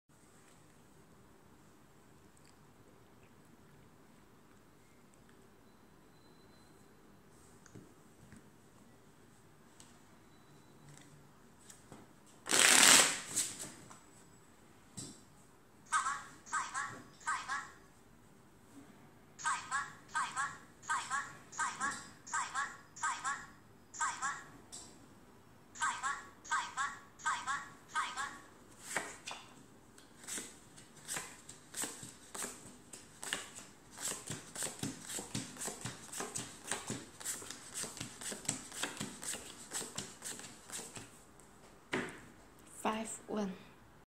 Latest Iphone poker scanner analyzer
This is the latest phone analyzer in the cover of an iphone, it looks well and work very well for games such as Texas and Omaha!